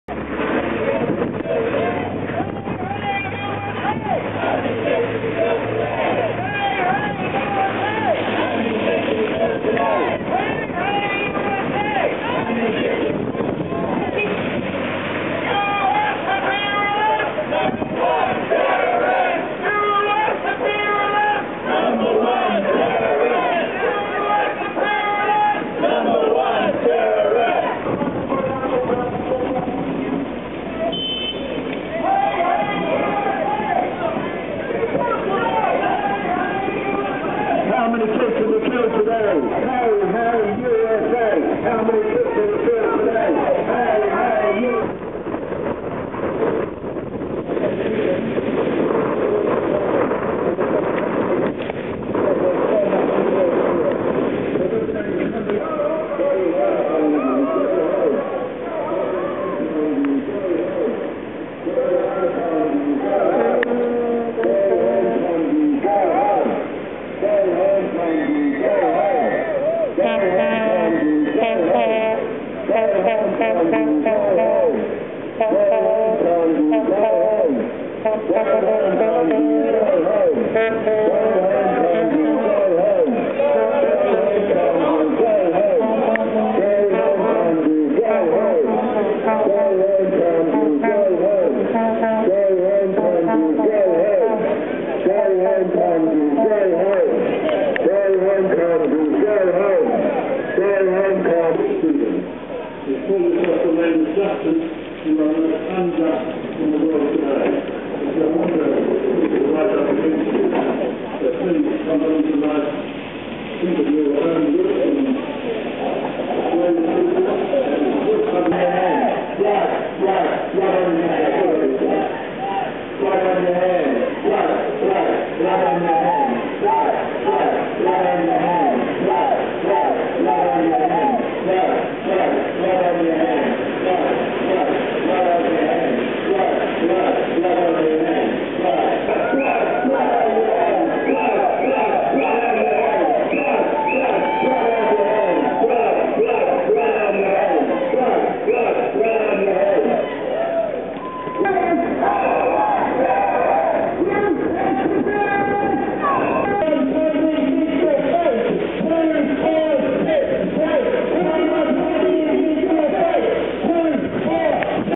Condi Rice's Visit to NZ, Part 1: Protest March. 26 Jul 08
Part 1:

Condoleezza Rice, the US Secretary of State, visited Auckland this weekend.

Here to greet her, in their usual warm friendly manner, was a crowd of activists who marched through an unusually strong "once-in-a-decade" storm from Auckland Domain to Government House, where "Condi" was meeting with NZ Foreign Minister Winston Peters.

 Held at bay by a large contingency of police, they protested loudly against US foreign policy, the war in Iraq, Guantanamo Bay, torture, imperialism, capitalism, etc, etc, etc. (Some of them also brought up Maori rights and others hurled personal insults at political figures, but I'm not sure how those were relevant or appropriate).

26 July, 2008
Park Road, Grafton, to
Mountain Road, Mount Eden